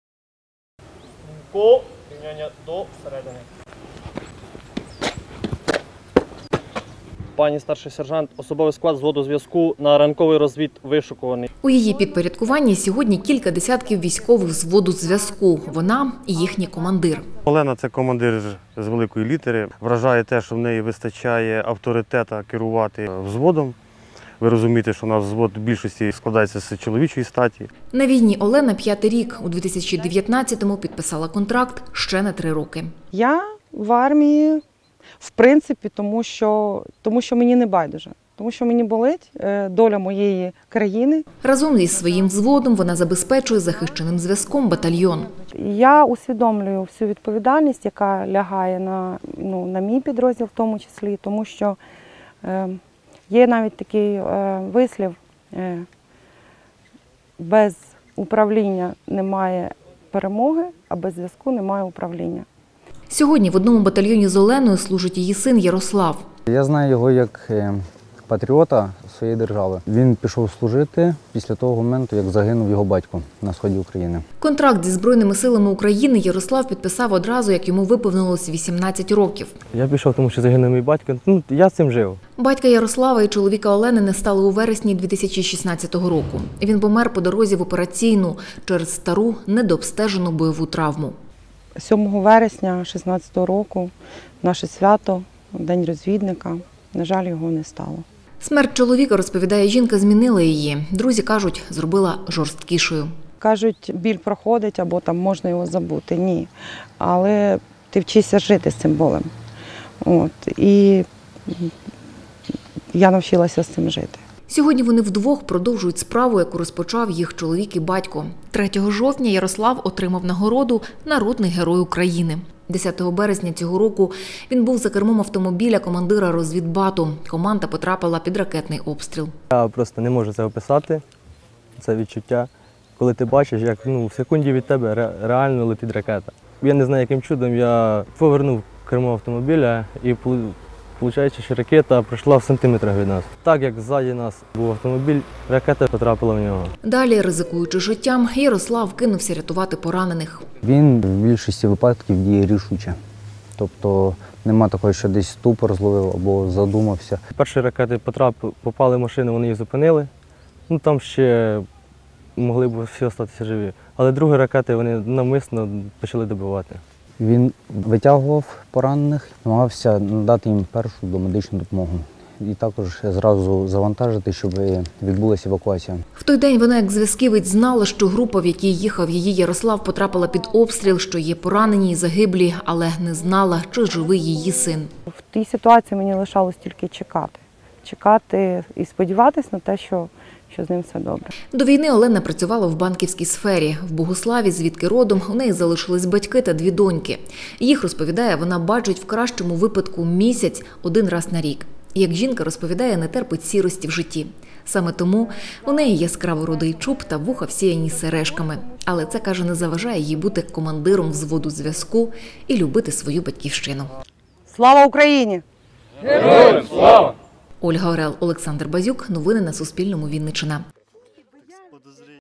14.10.2020. День захисника України: історія сім’ї розвідників
Чоловік пішов захищати Україну у 2015 році, вона у 2016, син – 2017. Всі у розвідувальній роту. Олена Кукла на війні 5-й рік. 4 роки тому війна забрала чоловіка: помер по дорозі в операційну через стару недообстежену бойову травму.